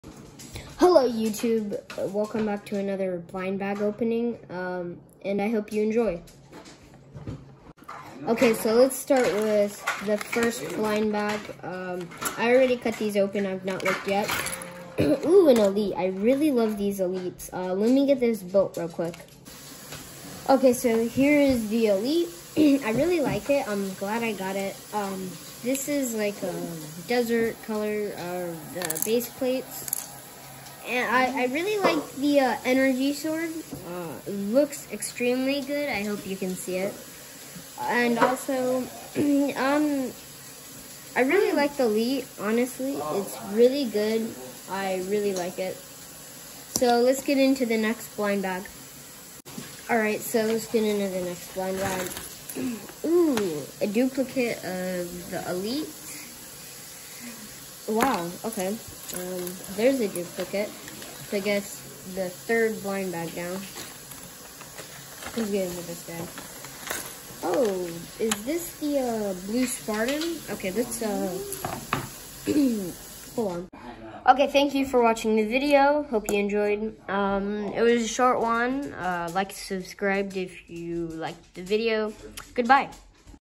Blind bag opening halo mega constructs
Opening three blind bags from mega constructs halo series